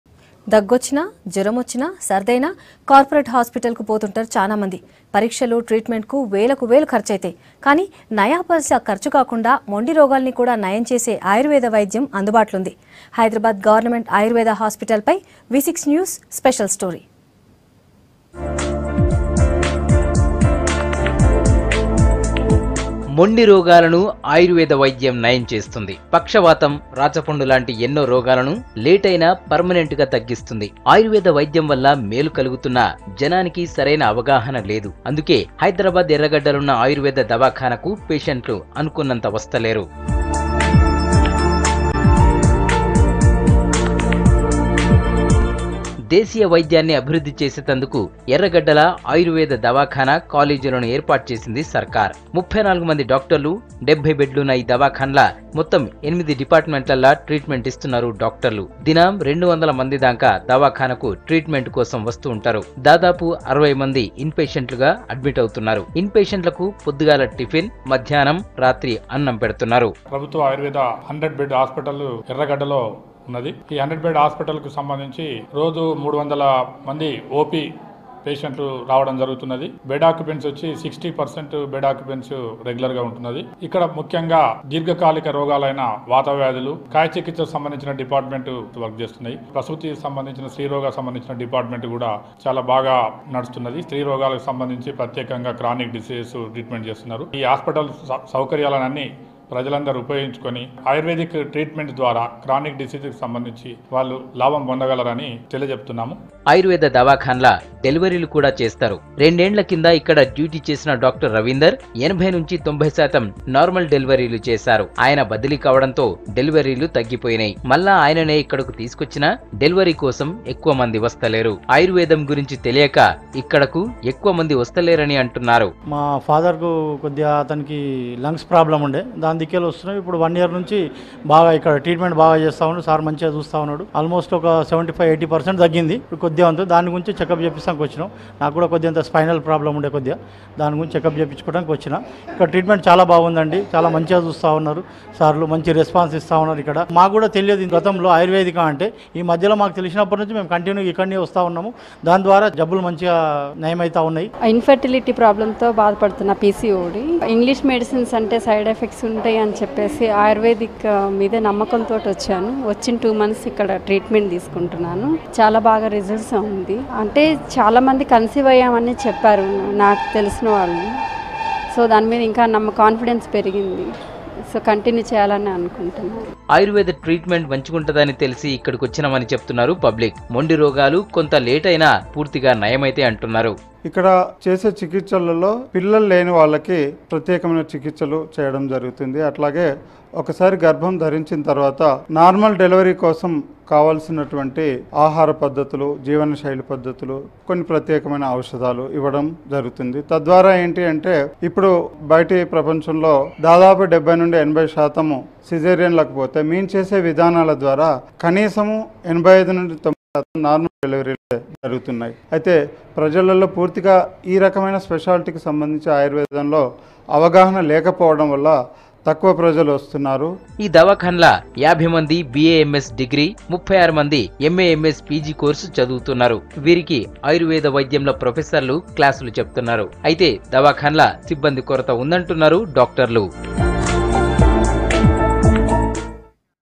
Special Story On Ayurvedic Govt Hospital In Erragadda | Hyderabad | V6 News
Special Story On Ayurvedic Government Hospital In Erragadda, Hyderabad.